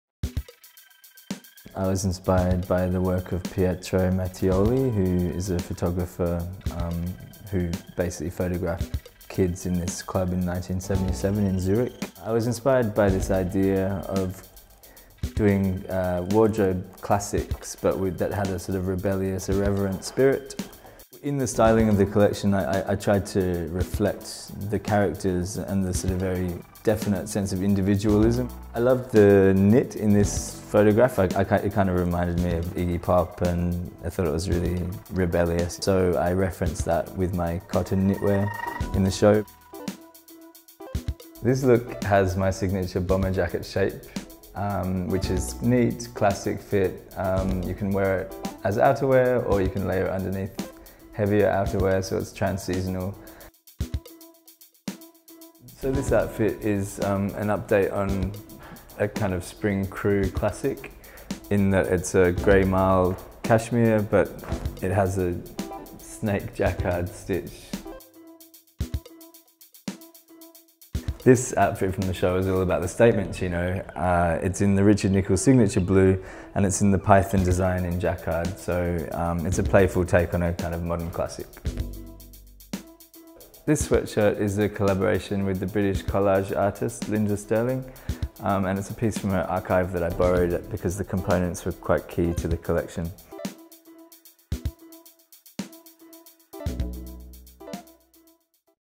Richard Nicoll Styles his Favourite SS14 Pieces | MATCHESFASHION.COM
Richard Nicoll styles his personal highlights from his SS14 collection of timeless yet special trans-seasonal pieces.